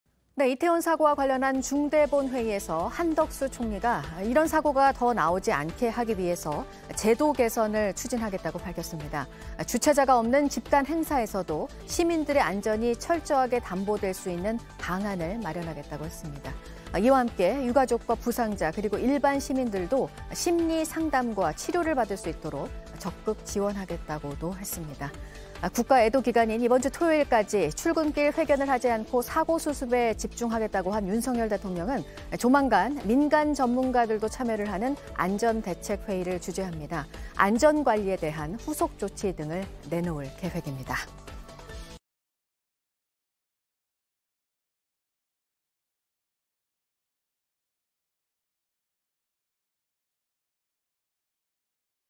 한덕수 총리 "유가족·부상자·일반 시민도 심리치료 지원" / JTBC News
이태원 사고와 관련한 중대본 회의에서 한덕수 총리가 이런 사고가 더 나오지 않게 하기 위해서 제도 개선을 추진하겠다고 밝혔습니다.

주최자가 없는 집단 행사에서도 시민들의 안전이 철저하게 담보될 수 있는 방안을 마련하겠다고 했습니다.

유가족과 부상자 그리고 일반 시민들도 심리 상담과 치료를 받을 수 있도록 적극 지원하겠다고도 했습니다.

국가애도기간인 이번 주 토요일까지 출근길 회견을 하지 않고 사고 수습에 집중하겠다고 한 윤석열 대통령은 조만간 민간전문가들도 참여하는 안전대책회의를 주재합니다.

안전 관리에 대한 후속조치 등을 내놓을 계획입니다.